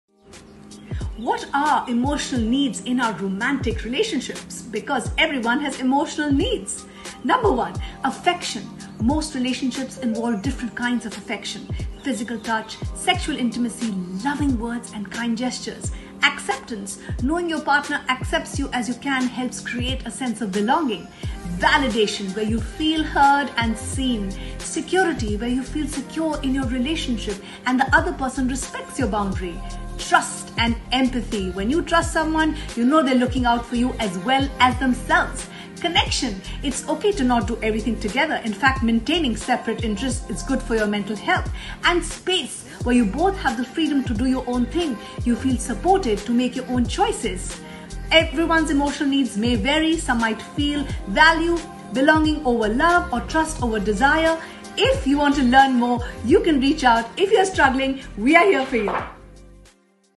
Emotional needs in a relationship - How to meet our needs ?
Everyone has emotional needs.

Consider basic survival needs like water, air, food, and shelter. Meeting these physical needs means you can stay alive, but it takes more to give life meaning.

You can’t see or touch things like companionship, affection, security, or appreciation, but they’re just as valuable. The same goes for feeling heard or valued.

In a relationship, the strength of your bond can make a big difference in whether you both get your needs met.

Although every relationship looks a little different, these emotional needs shared in our video are a good starting point for considering whether you and your partner are each getting what you need from the relationship.